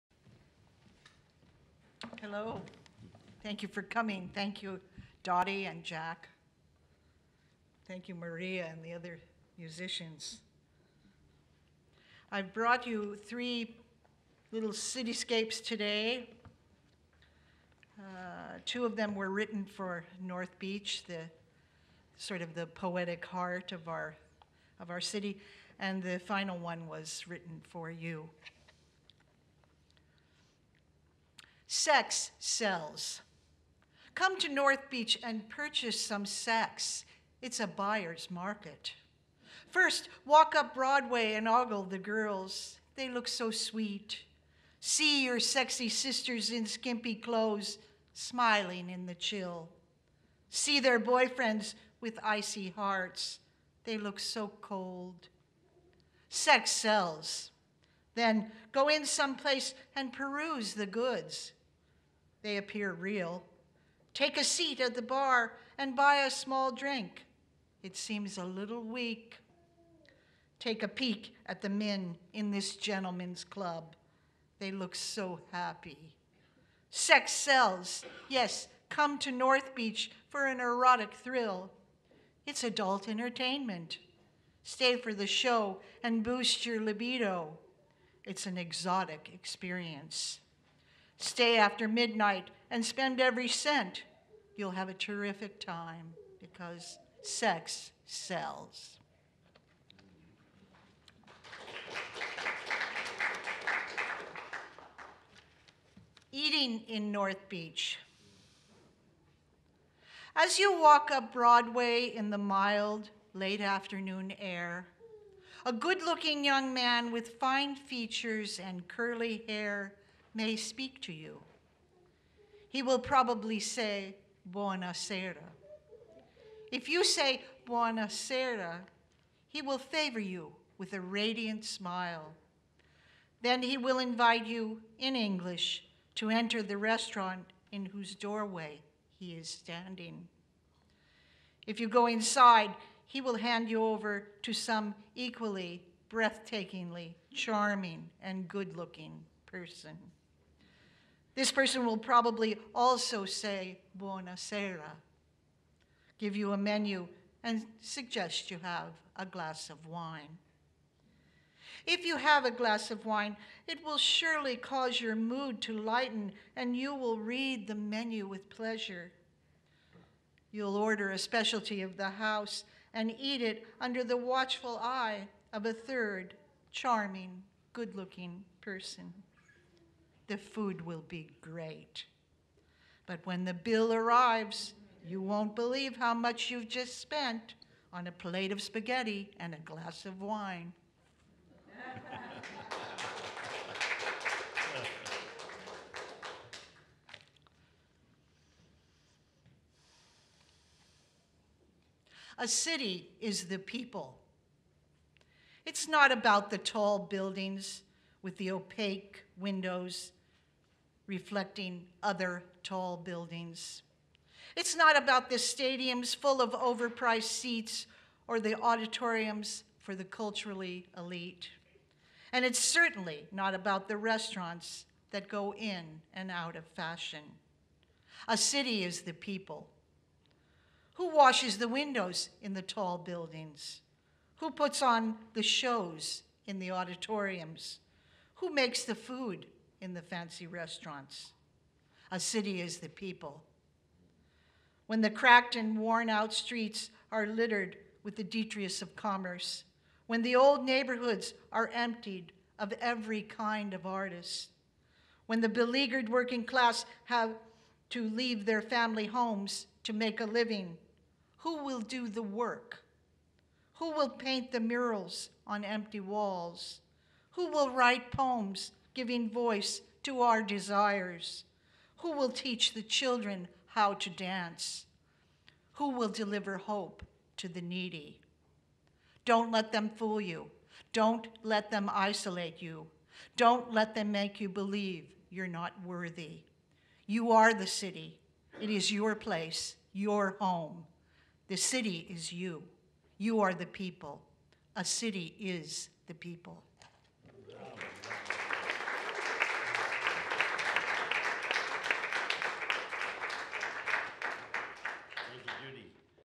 Judith Ayn Bernhard at the San Francisco Public Library
"This event features poets and musicians celebrating the diversity that makes San Francisco great. Featured poets are Judy Bernhard, Jack Hirschman, Genny Lim, Agneta Falk, Dorothy Payne and Alejandro Murgia, Mahnaz Badihian, Maria Medina Serafin, Jorge Molina, Dorothy Payne, Toshi Washizu, Sarah Menefee, Francisco Orrego and musicians Francisco Herrera, Claudio Santome and Son de la Mission! Featuring Maria Medina, Colin O’Leary, Francisco Ferrer, Garsha Shabankhani and more!